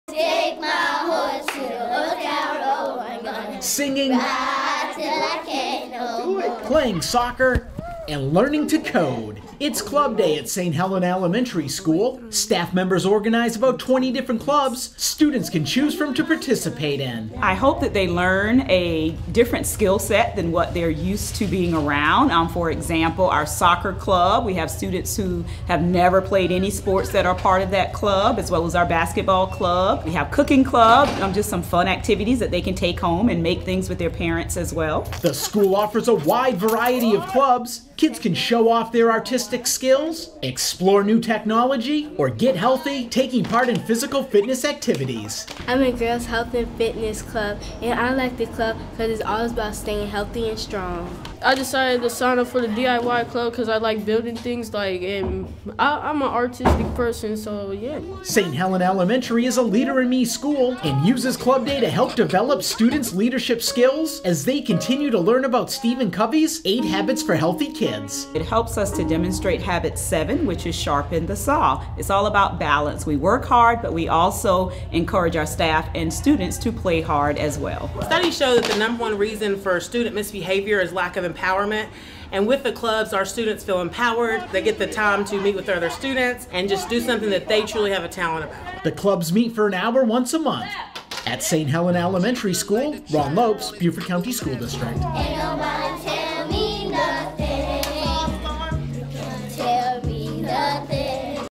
Club Day Fun
Students at St. Helena Elementary School take part in Club Day.